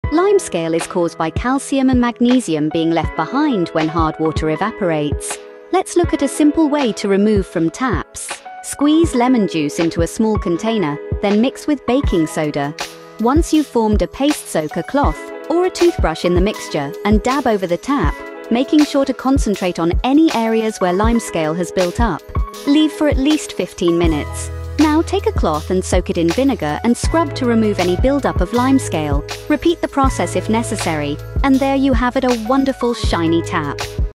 Limescale, remove it the natural way!
How to remove limescale from taps naturally with just lemon, baking soda, and vinegar. Rub lemon and baking soda to dissolve limescale, and finish with vinegar to bring a sparkle. It's easy, eco-friendly, and leaves your tap sparkling clean!

Links
Sodium Bicarbonate - 5KG
Jocker Woods White Vinegar for Cleaning